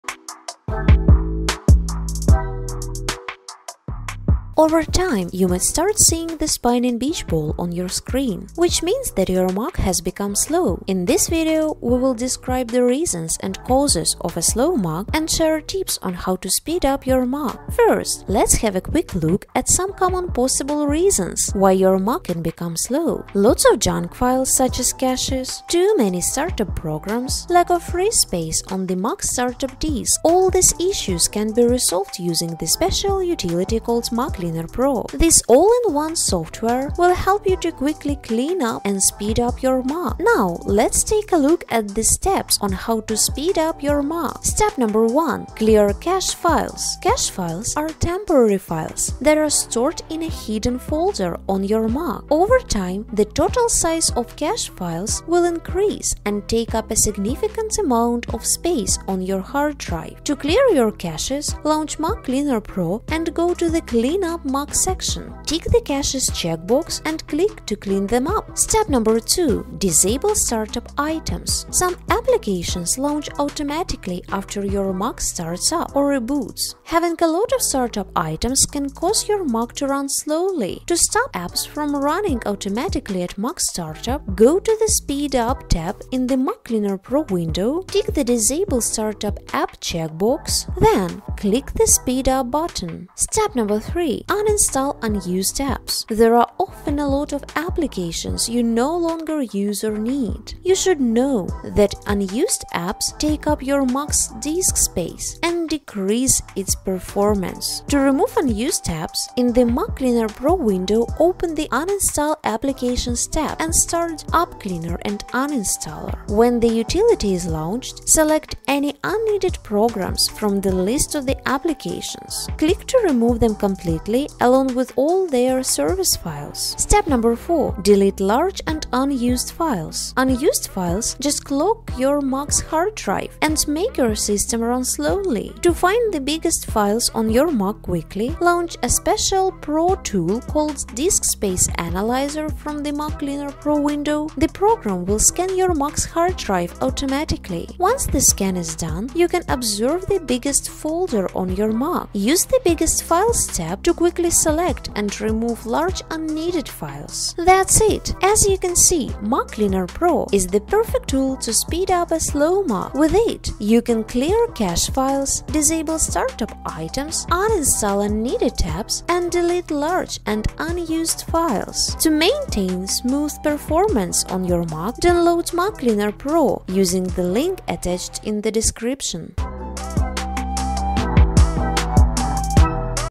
How to speed up a slow Mac
Over time you may find your Mac running slow. There may be various reasons for a slow Mac but the most common is a lack of free space on the disk. In this video, we will share the reasons why you have a slow Mac and steps on how to fix poor Mac performance.

00:00 - Intro
00:20 - Reasons why your Mac can become slow
00:47 - Clear Cache files
01:09 - Disable startup items
01:34 - Uninstall unneeded apps
02:08 - Delete large and unused files